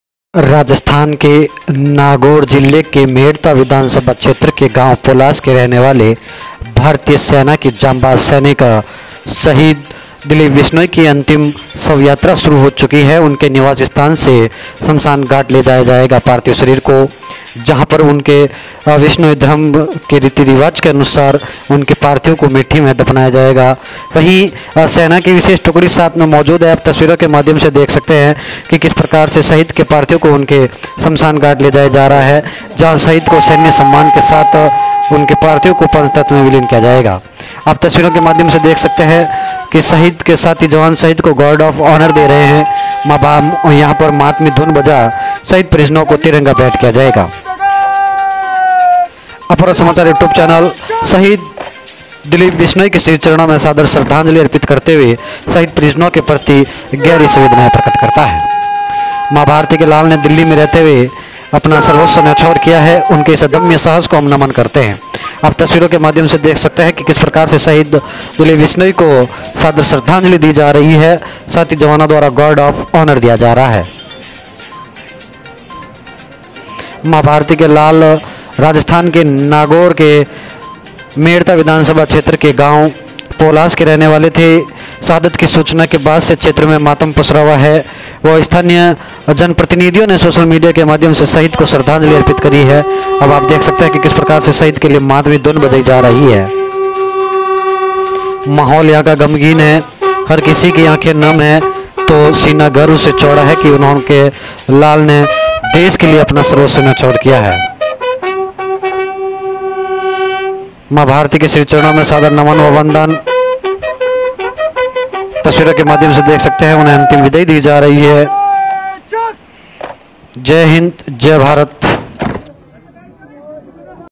शहीद दिलीप बिश्नोई जी ने महज 22 वर्ष की उम्र में दी शहादत,अंतिम विदाई में उमड़ा जनसैलाब
🇮🇳नागौर ग्राम shahidarmy पोलास निवासी भारतीय सेना में कार्यरत श्री दिलीप जी विश्नोई के शहीद होने की खबर से मन आहत है। भगवान उनकी आत्मा को शान्ति प्रदान करे, साथ ही उनके परिवार को दुःख सहन करने का संबल प्रदान करे! ॐ शांति!!